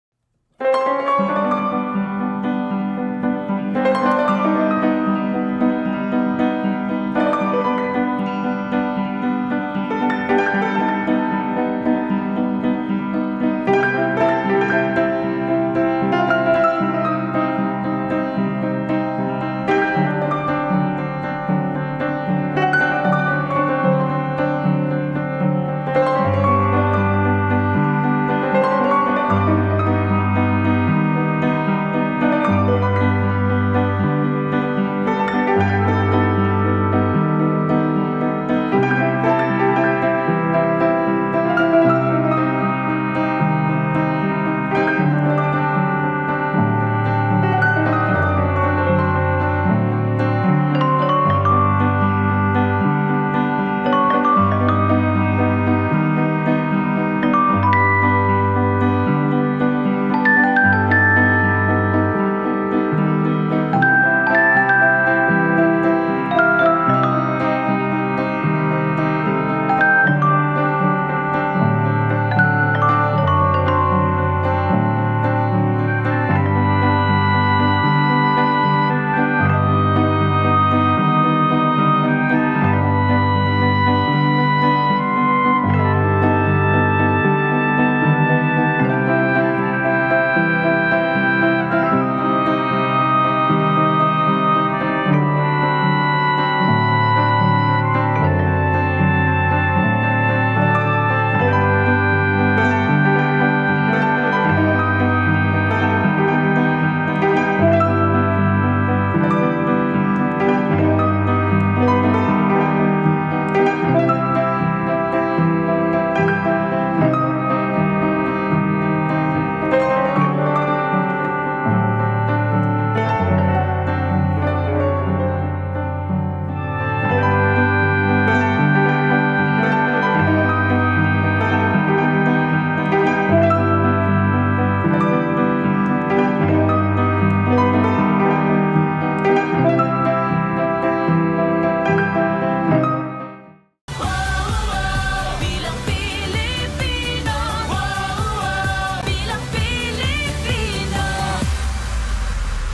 Atty. Larry Gadon, sinuspinde ng SC dahil sa verbal assault sa isang journalist
Sinuspinde ng Korte Suprema si senatorial aspirant Atty. Larry Gadon matapos magbitiw ng masasamang salita laban sa journalist na si Raissa Robles. Tingin naman ni Gadon, politically motivated lang ito dahil tumatakbo siya bilang senador.